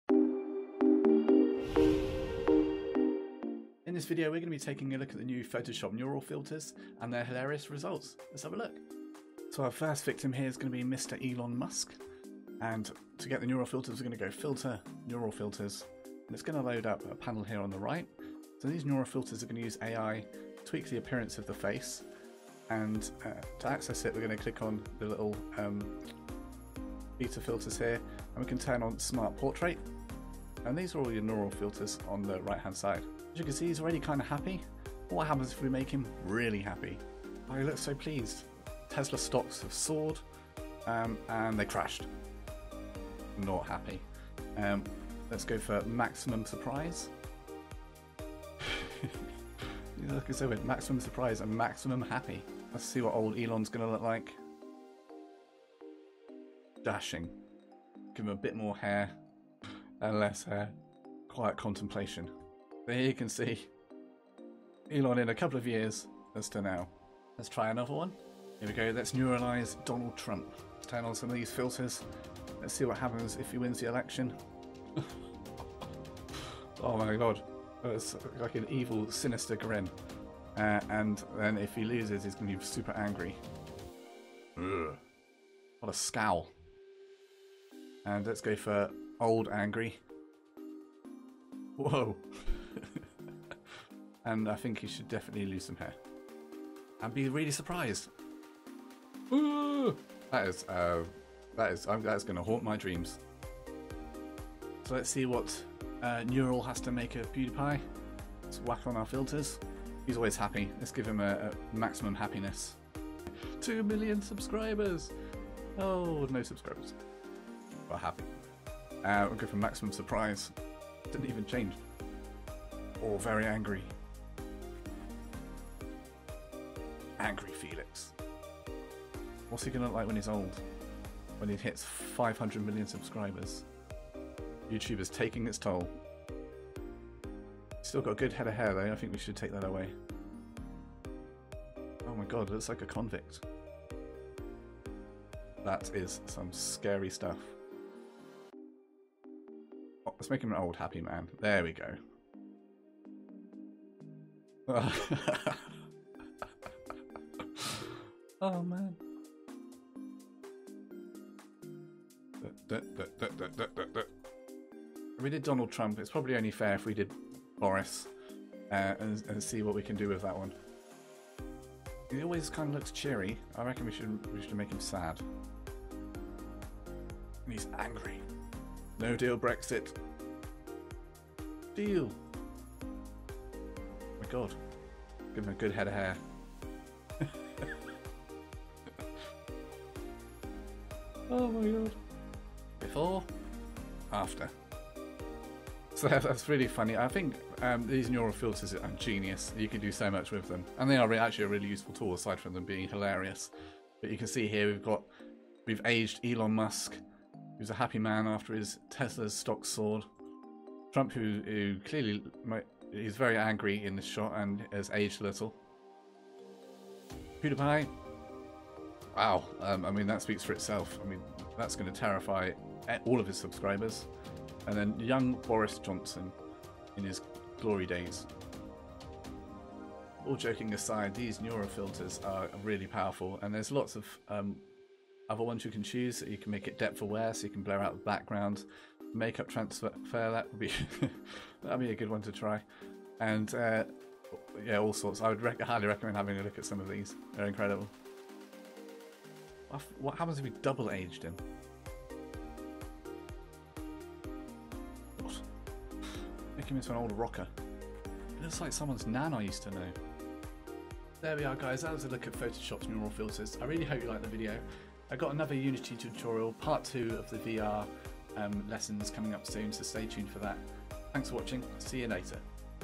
PEWDIEPIE GETS OLD! - The Power of Neural Filters
Photoshop released an update a few days ago, part of that update included the new Neural filters that use AI to change the appearance of faces... funny stuff! take a look as I mess about with some famous faces, Elon musk, Pewdiepie, Donald Trump & more

Social Stuff!